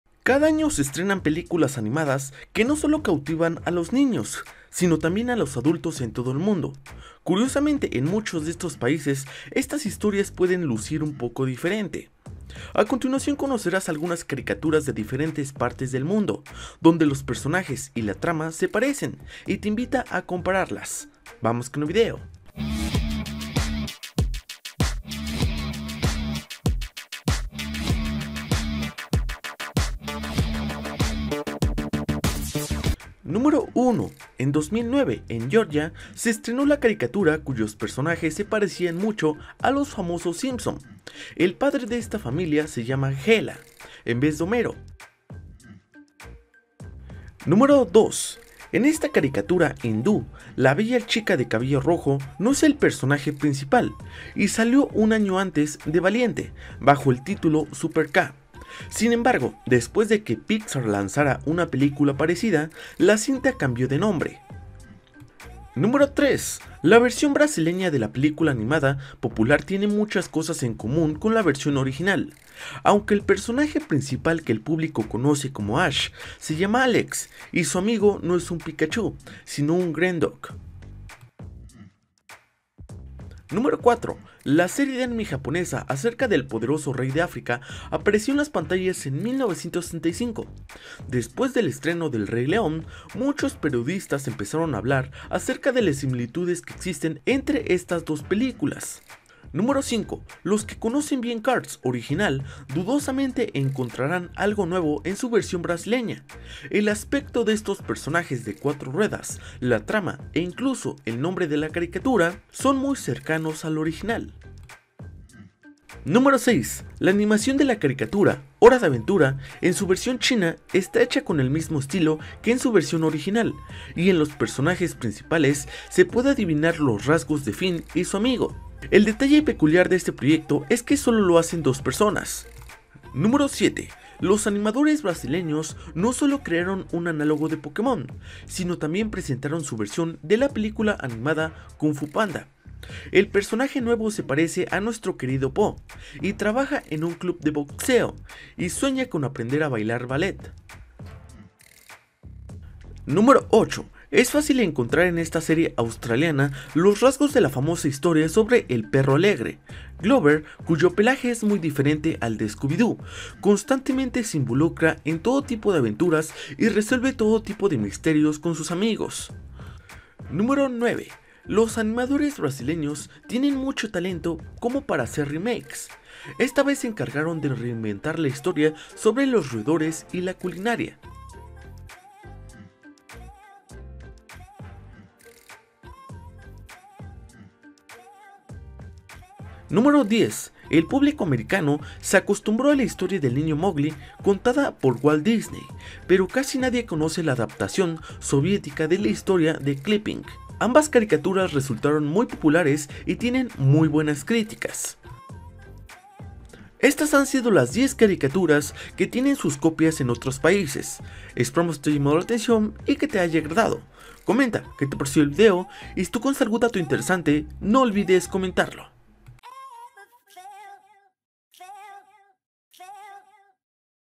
Las 10 Copias Más Descaradas de las Caricaturas
Cada año se estrenan películas animadas que no solo cautivan a los niños, sino también a los adultos en todo el mundo. Curiosamente, en muchos de estos países estas historias pueden lucir un poco diferente.
A continuación conocerás algunas caricaturas de diferentes partes del mundo, donde los personajes y la trama se parecen, y te invita a compararlas.